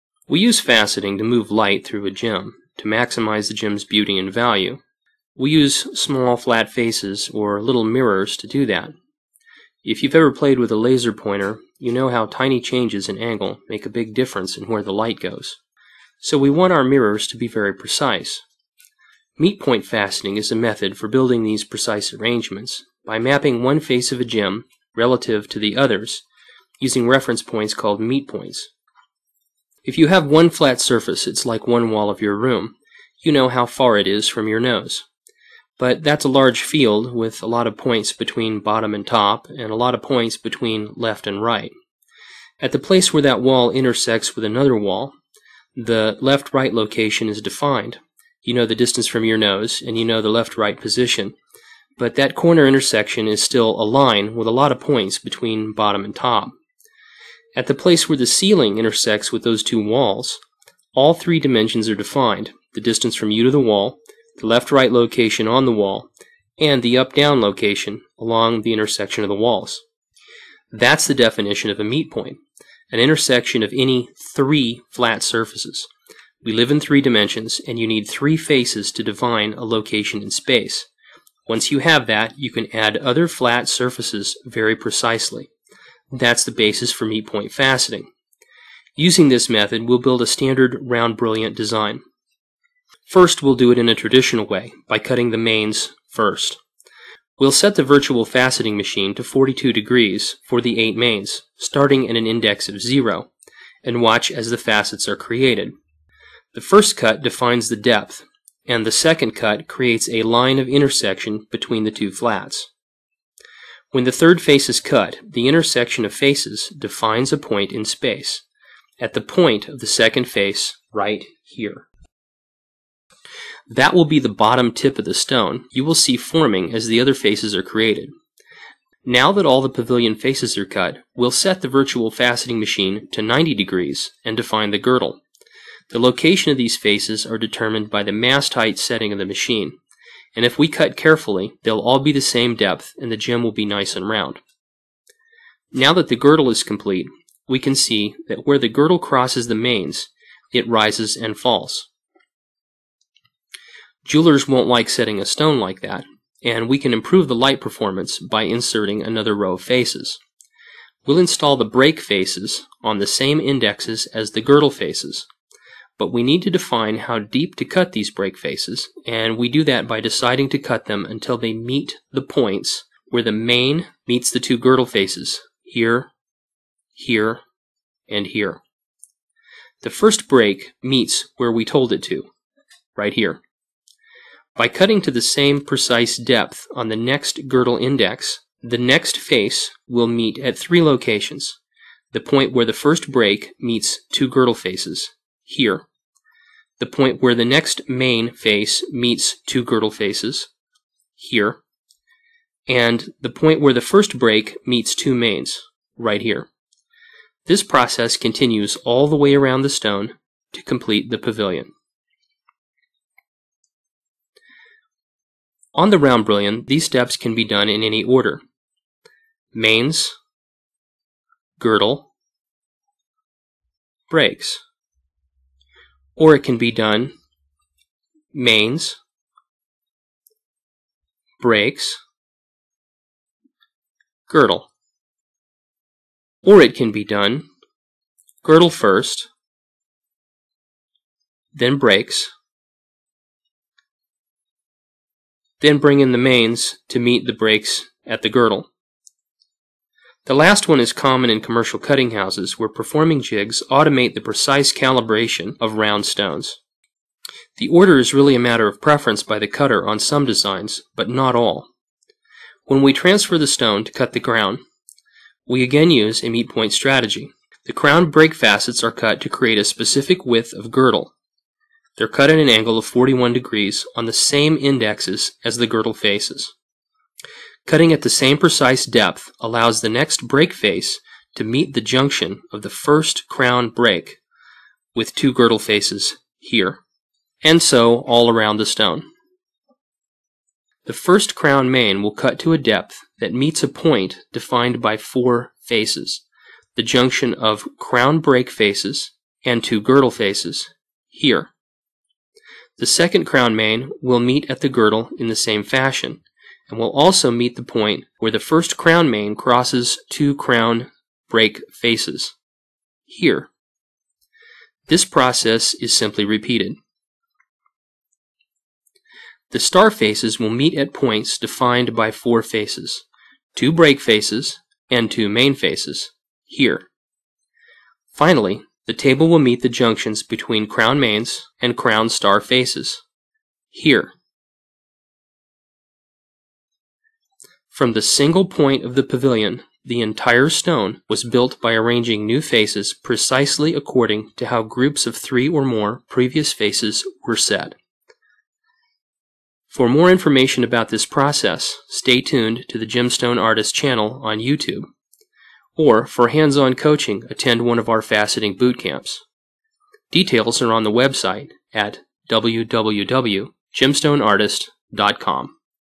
Meetpoint Faceting #1 - the SRB
Quick introduction to meetpoint faceting concepts with the Standard Round Brilliant Design.